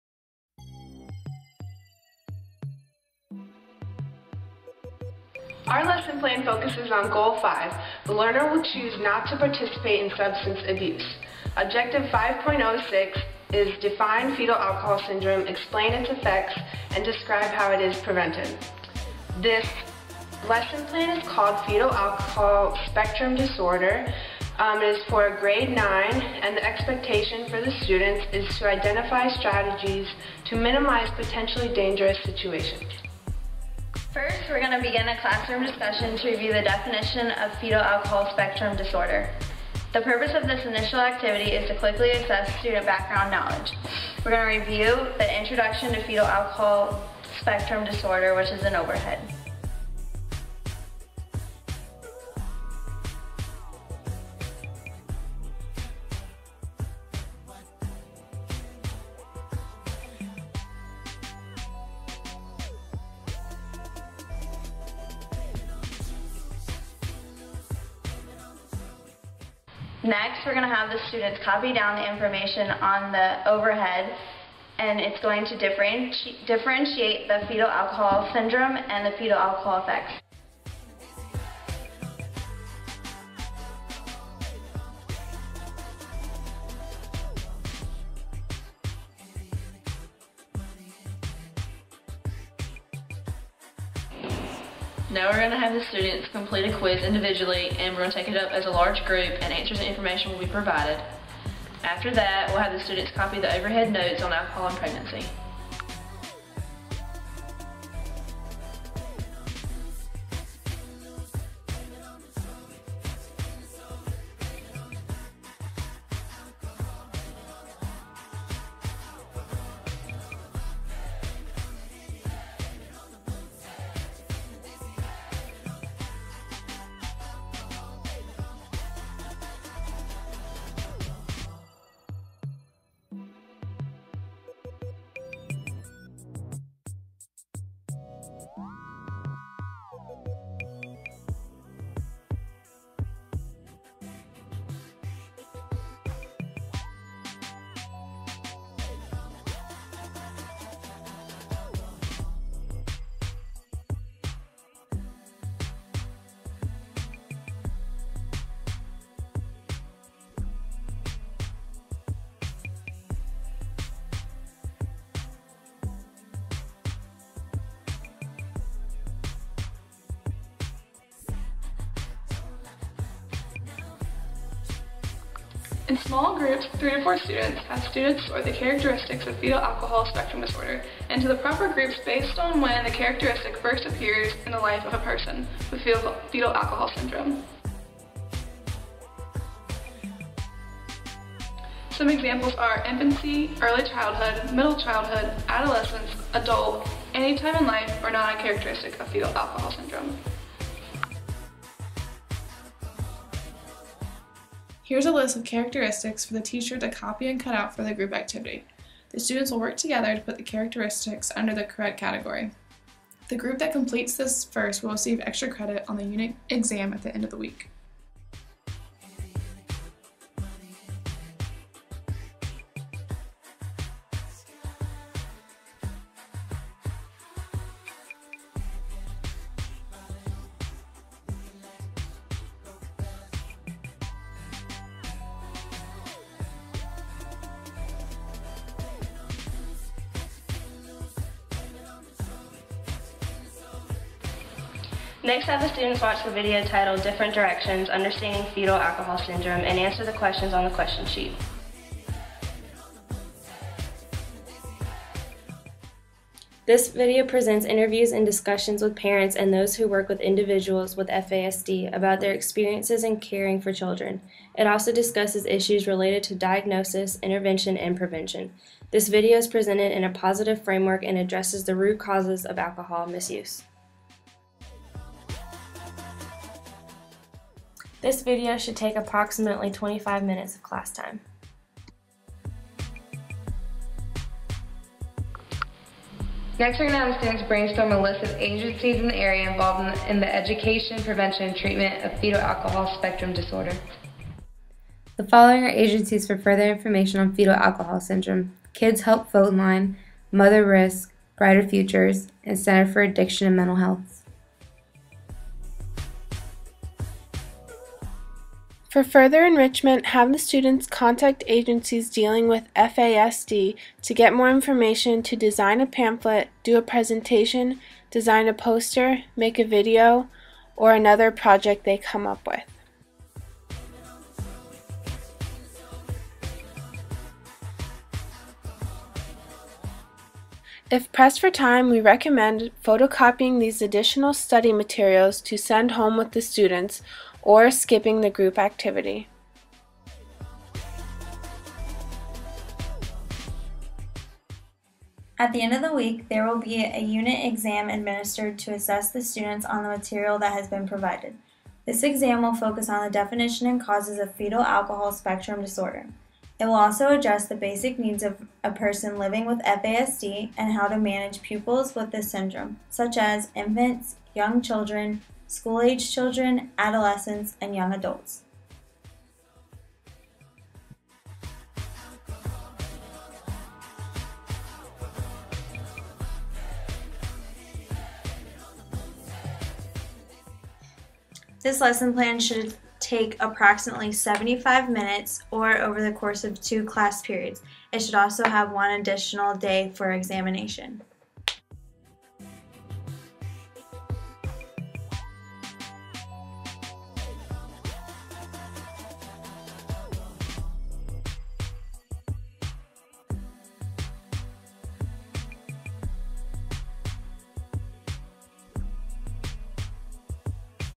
Fetal Alcohol Spectrum Disorder Lesson Plan
Here is our group's Lesson Plan presentation for HLTH 3355: Alcohol, Tobacco, and other drugs Education and Prevention. This lesson plan focuses on Fetal Alcohol Spectrum Disorder and contains different methods of teaching the material to high school students, specifically 9th grade. The group members consist of: Lindsey Dawson, Liz Logue, Jazmine Munoz, Morgan Stott, Hilary Todd, and Ashley Yelmini.